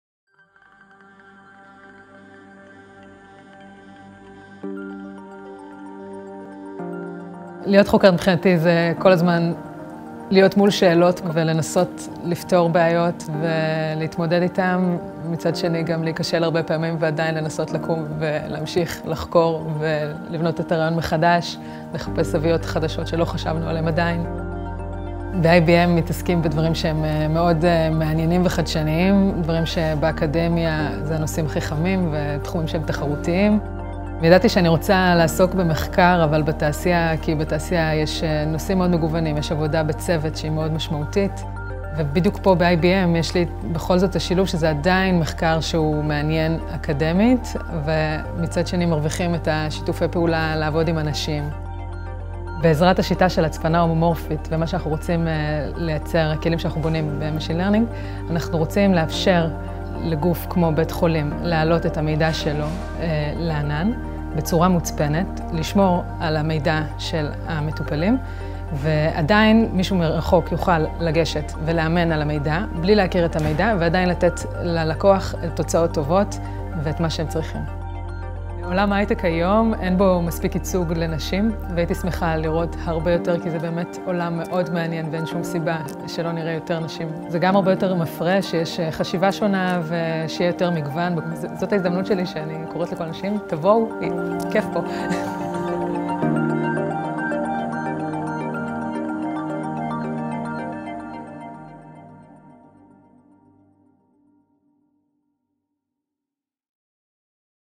AI and fully homomorphic encryption at IBM Research - Haifa
Moran Baruch, machine learning and cybersecurity researcher at IBM Research – Haifa, explains how her team uses homomorphic encryption and machine learning to provide better security for cloud-based data so it can be analyzed without being exposed. Moran loves being part of a team that invents new technology, and hopes to see more women join her in the IT field.